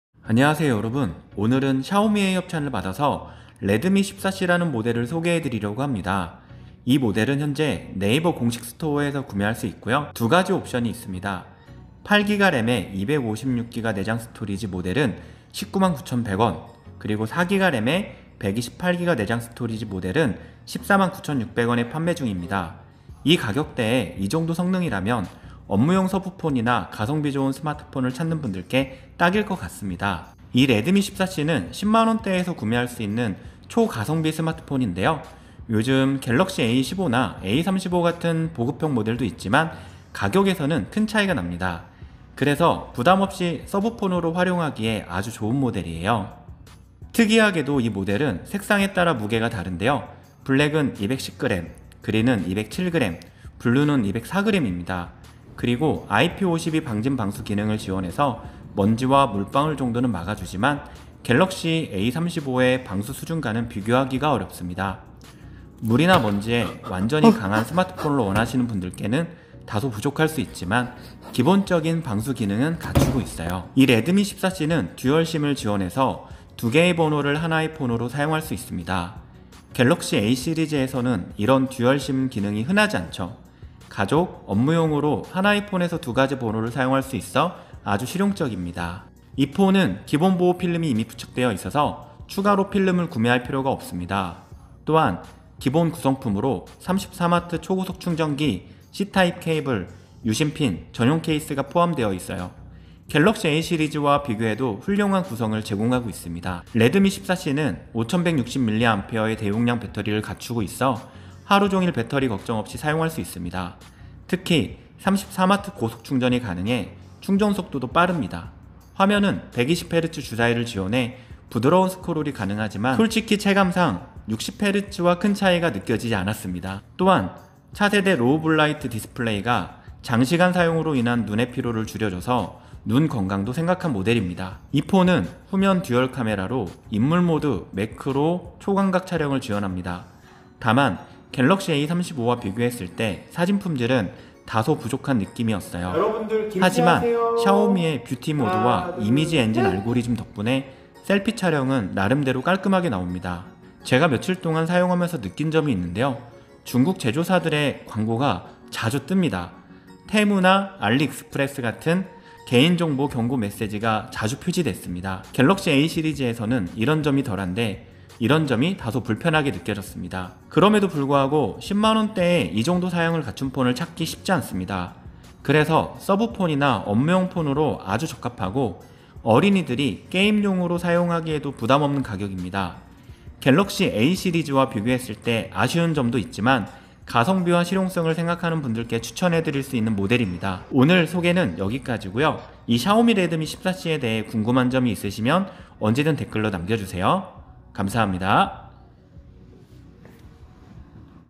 진짜 레알 10만원대 폰 입니다 ㅋㅋ 샤오미 레드미 14C
19년째 SK텔레콤 공식대리점을 운영하면서 약 3만대에 가까운 폰을 직접 판매해온, 폰킴의 진심어린 리뷰!
오늘은 샤오미의 레드미 14C 드루와!!!